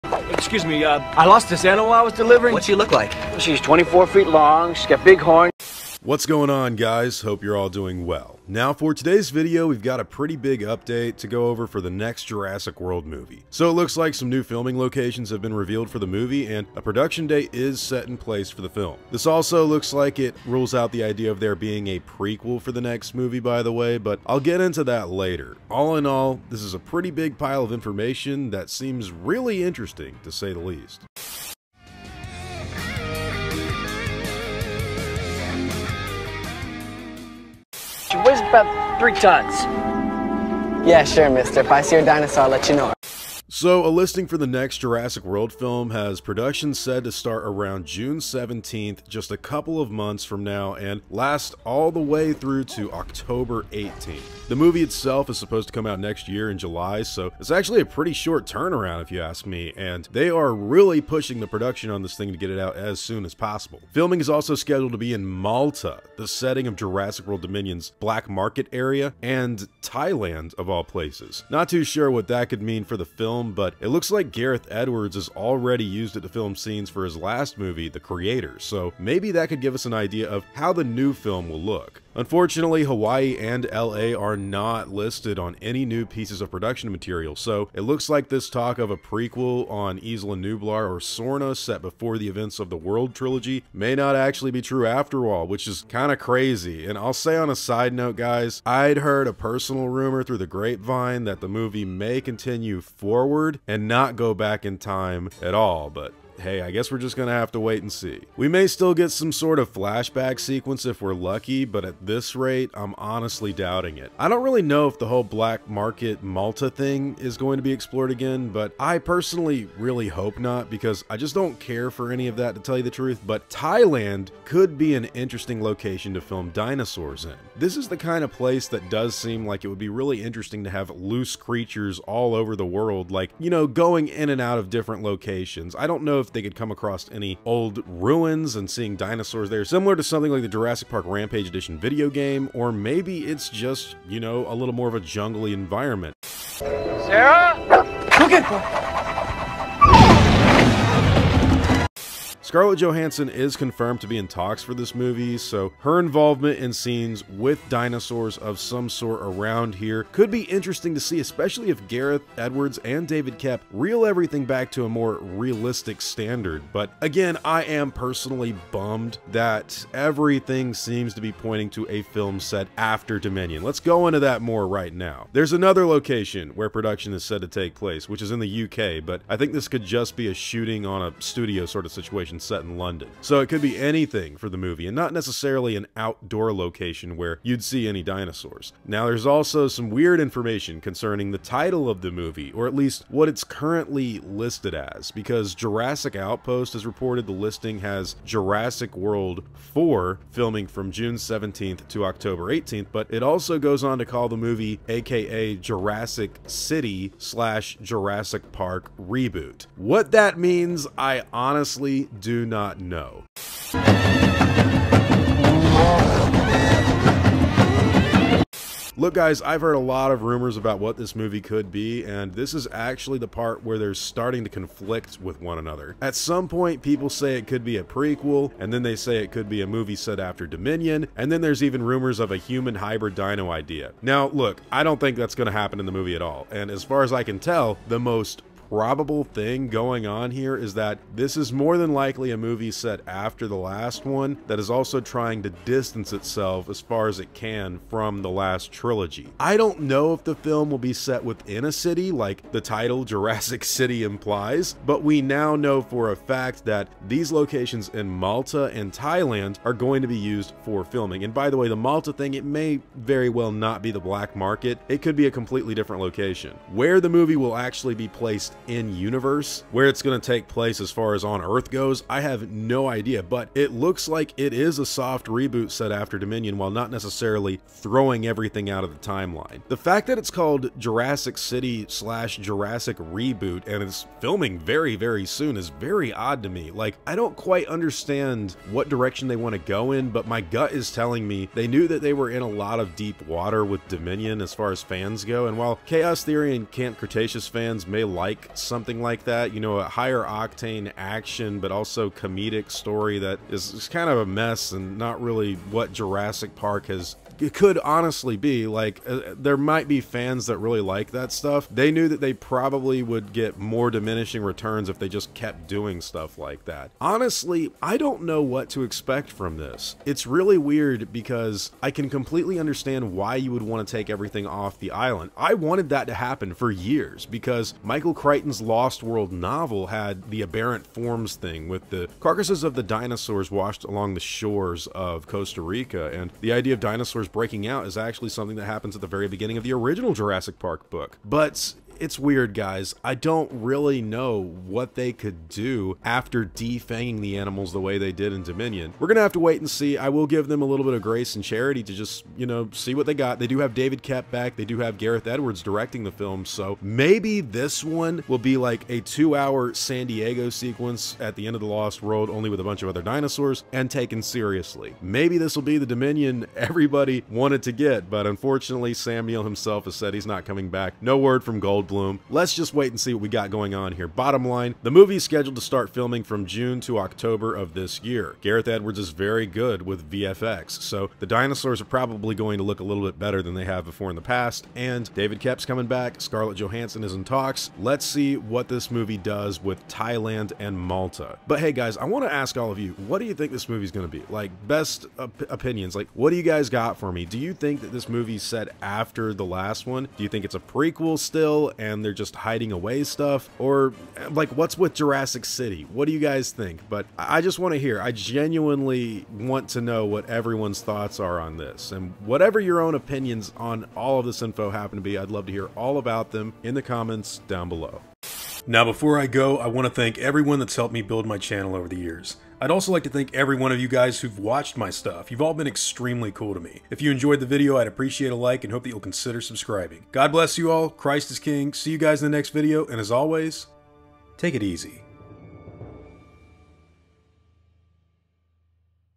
New Update On Jurassic World 4 - Filming Locations Revealed
Jurassic World 4 has David Koepp and Gareth Edwards joining the dinosaur saga but the new filming info reveals some stuff that is kind of odd with the new movie. A lot of rumors about it being a reboot and set in a city have come out and it looks like it may not be a prequel after all. So far everything we know about the movie is minimal.